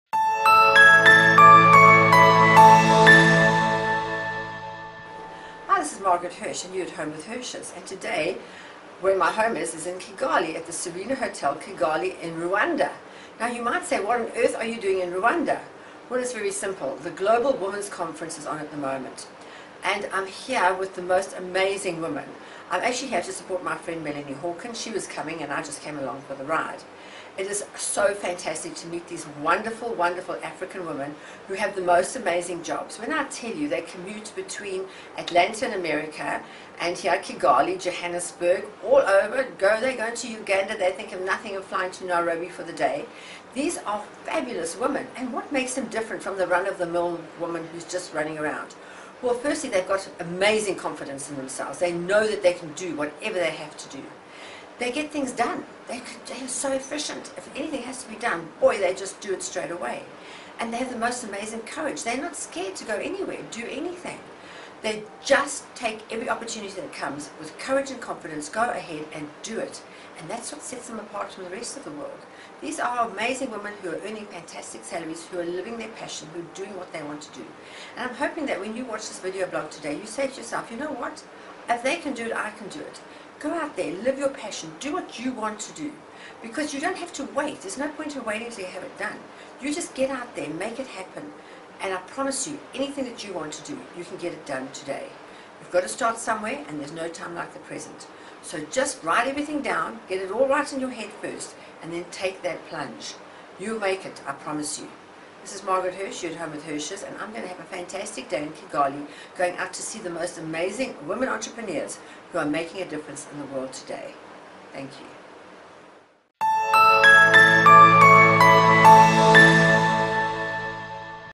How to get things DONE! by Margaret Hirsch
Last night was the Women's conference in the beautiful Rwanda and the 1 thing that stands out from all the incredible women I have met here is that they all have such courage & confidence that they can "Just get things done!"